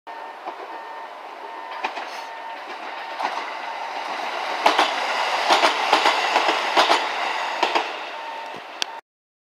★20191214 キハ75形 ＠名古屋駅付近４
撮影路線：JR東海 関西本線
撮影場所：愛知県名古屋市中村区長戸井町１丁目（向野橋）
行先：快速みえ 名古屋
車両：キハ75形
両数：2両
撮影機材：【ビデオカメラ】JVC GZ-RX680
MH：ーー
備考欄：